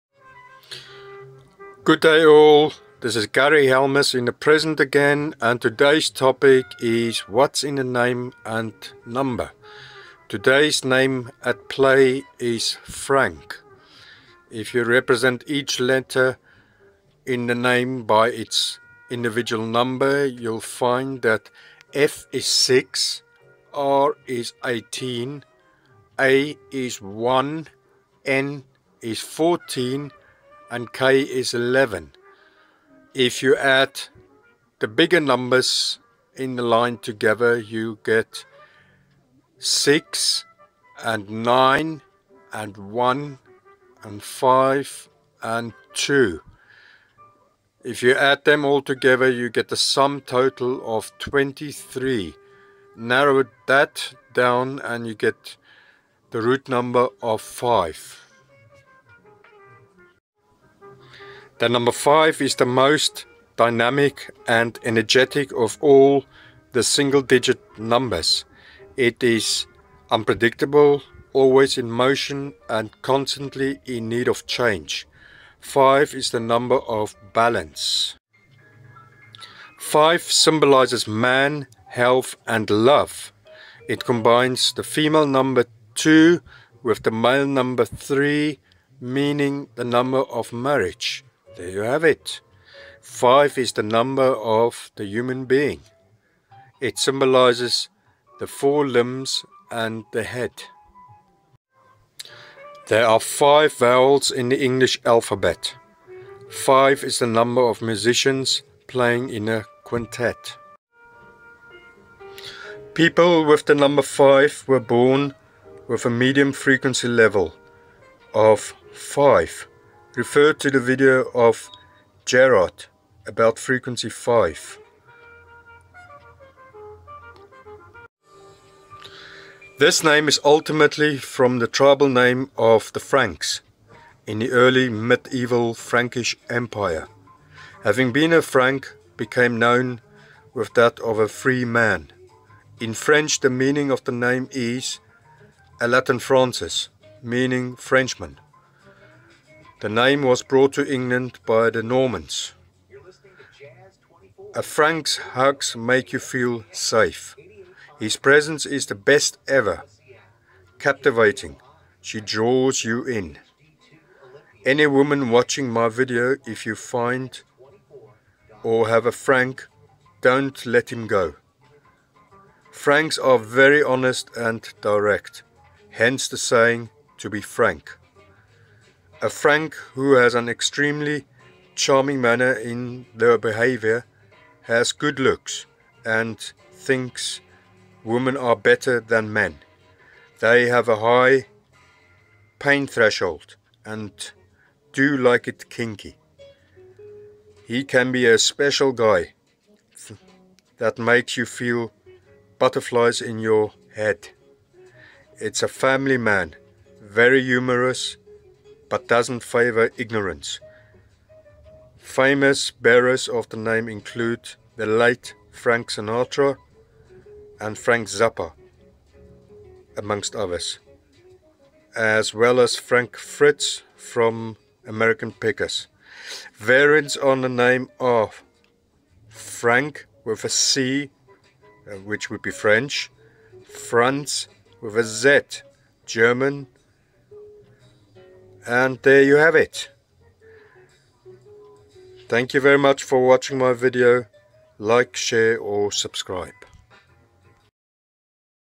What's in the name and no.! Frank?
A brief description of the name, Frank and it's rootnumber!